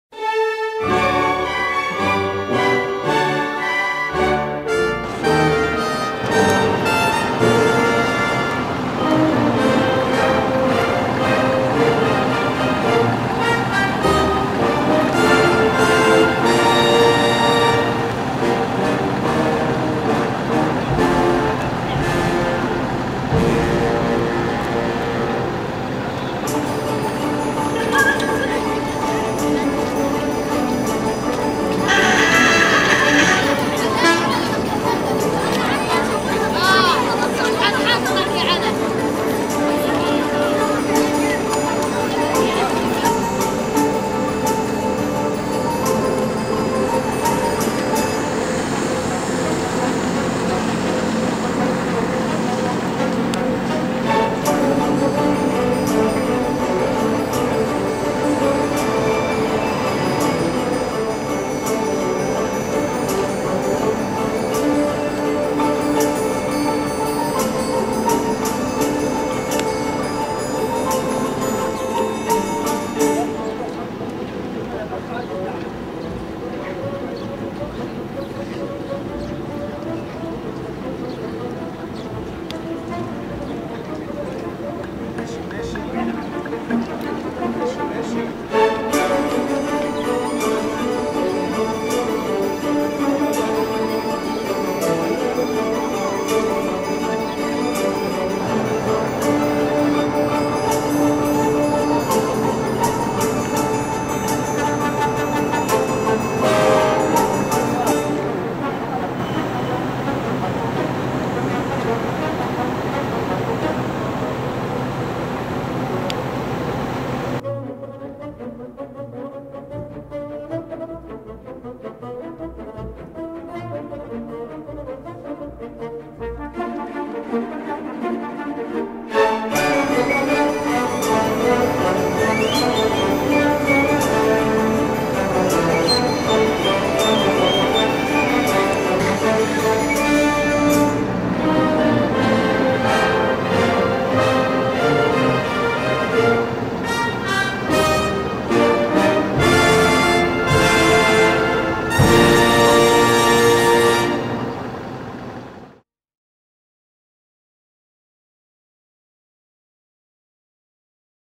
Temple of Hatshepsut Luxor Egypt
Temple of Hatshepsut Luxor Egypt, the revolutionary concept employed by Senmut, architect and prime minister of the queen Hatshepsut.